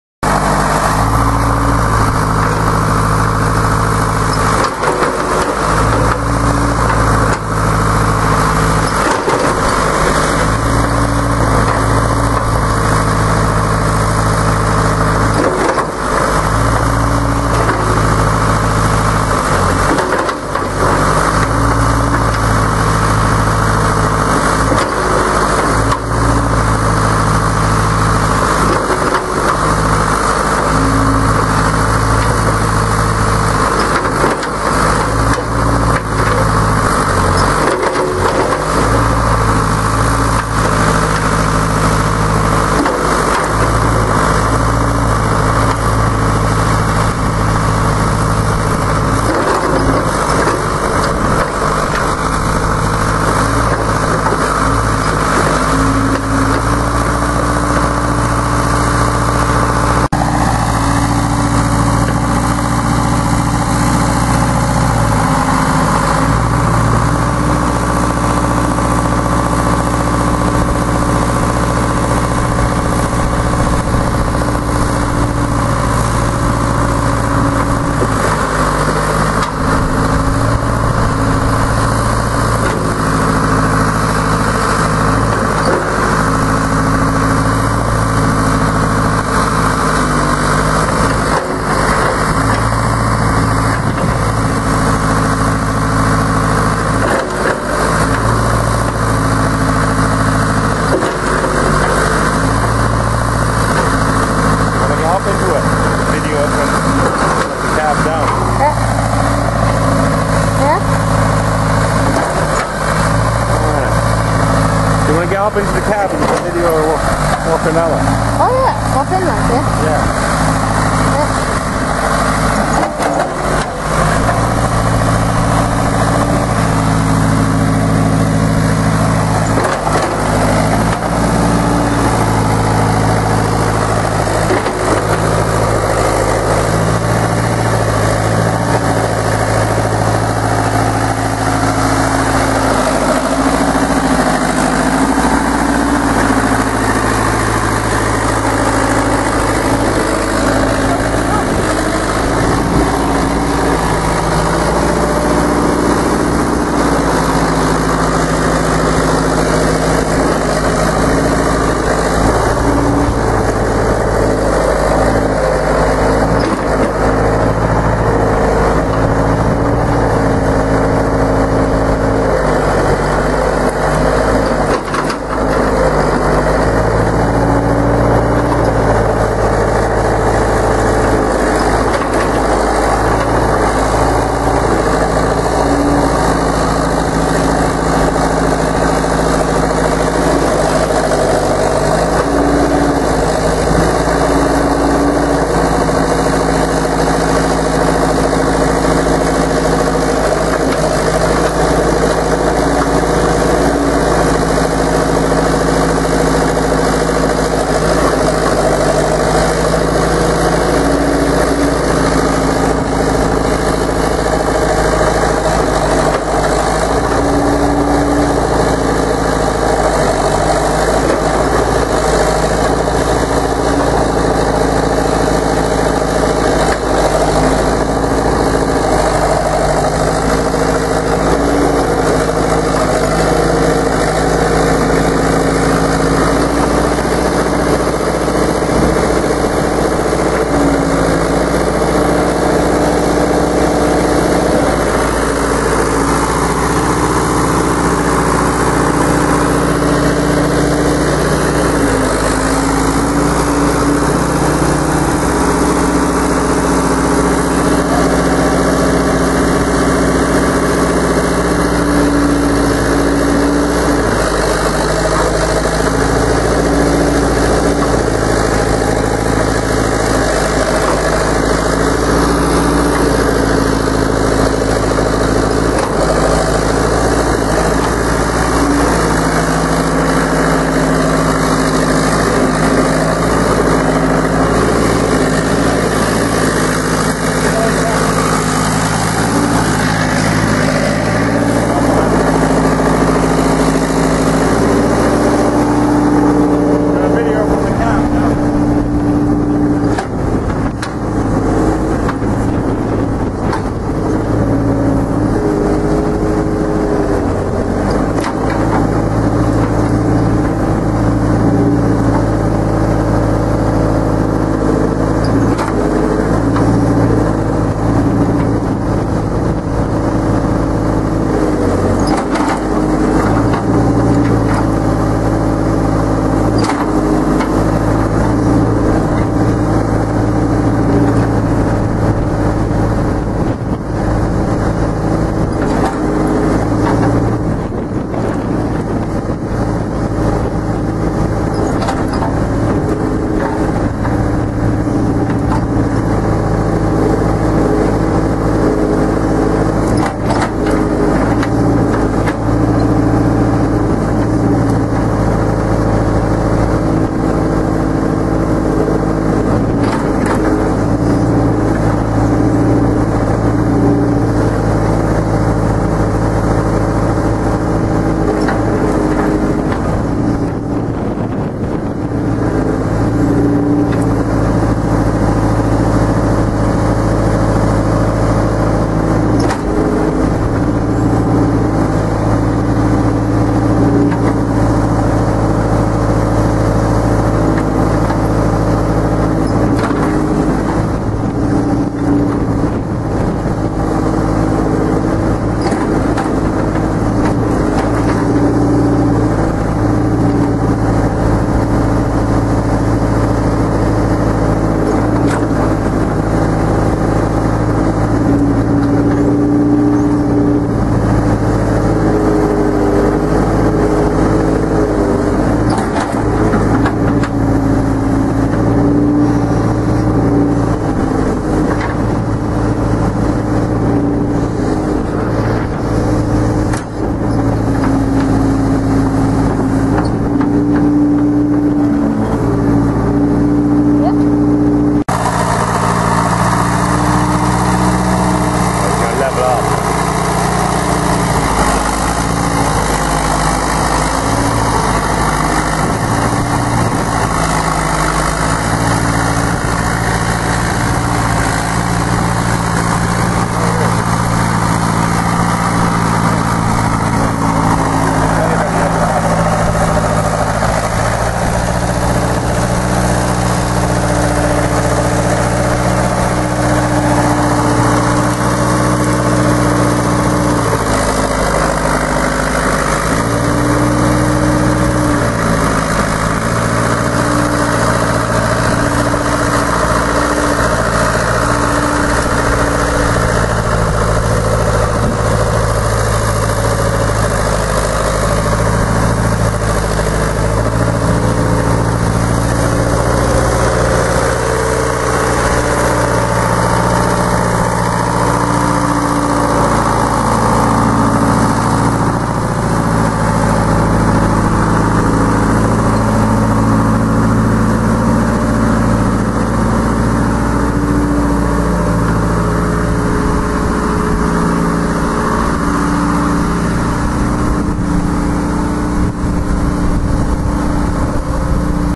hy-mac 570 1974 digger
is this the last one still working in ireland one owner from new